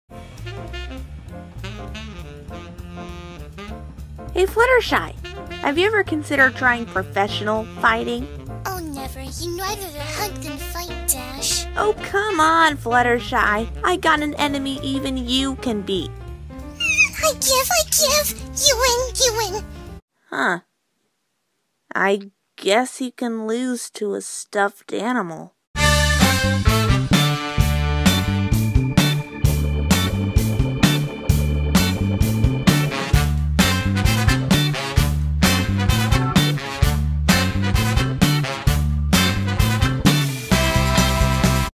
"Ultimate Fighter" MLP comic dub(Month of Lurve)
It ain't romantic but I think flutterdash counts right since those two are there XD
MLP: FIM is owned by Hasbro
Cast:

Fluttershy- VanillaShine357

Rainbow Dash- Eliza Vandewalle